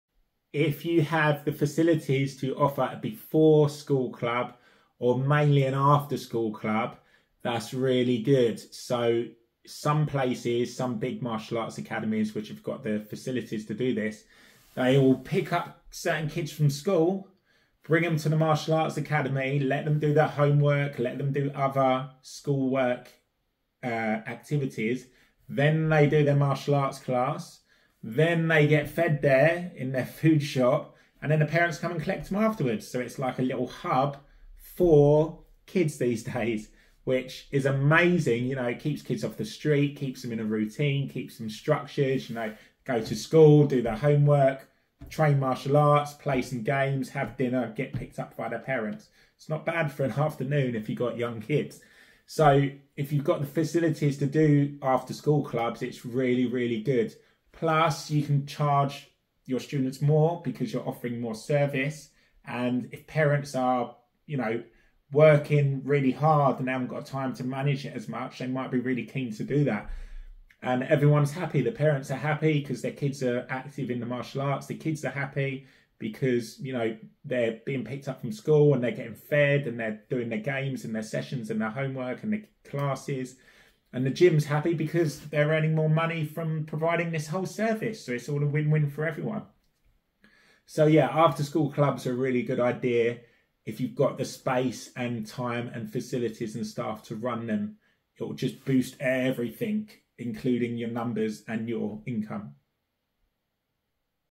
Martial Arts Business - 65 - Academy - Before and After School Clubs
Providing before and after school clubs at your Martial Arts School will bring more value and experience to families in your area.